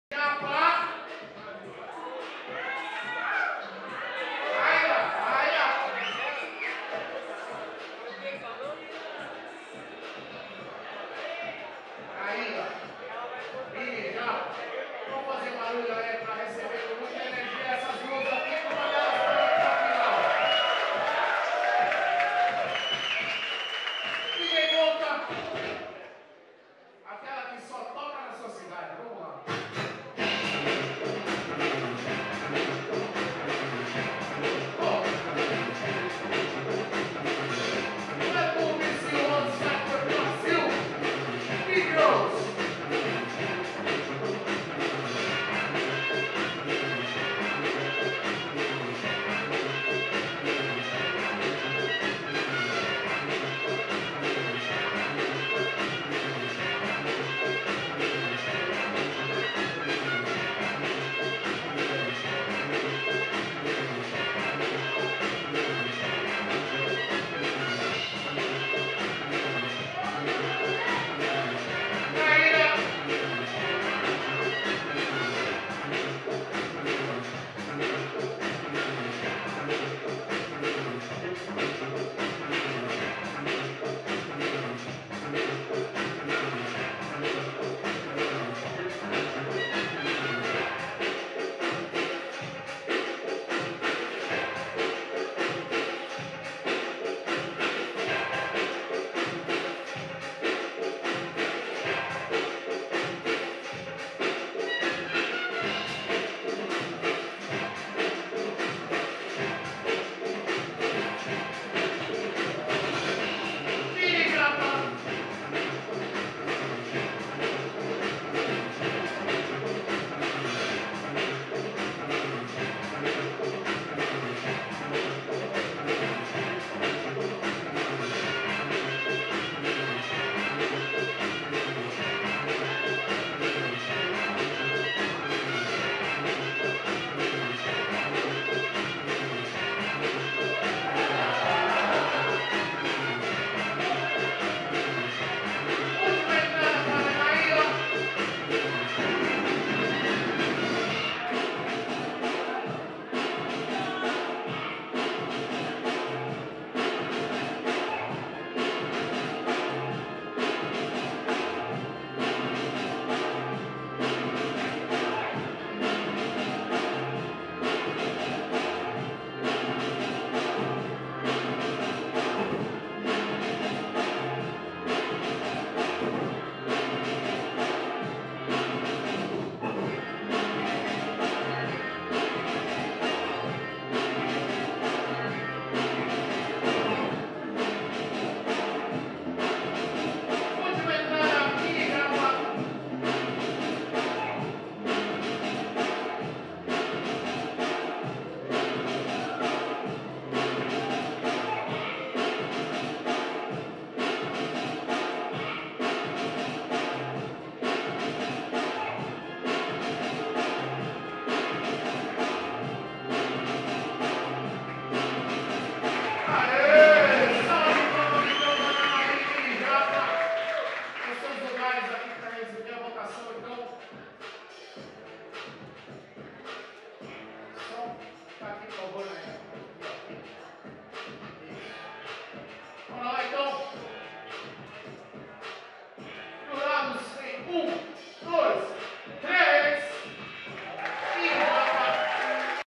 Naira vs Mini Japa - Semifinal - Red Bull BC One Cypher Bgirls - BreakSP Battles
-[ SETUP ]-
Lenovo G40-70 (Intel i3, 4GB RAM, 1TB HD)
Sony Handycam Cx220
Canon t5i - Lente 18-55mm e 50mm
Microfone Direcional Azden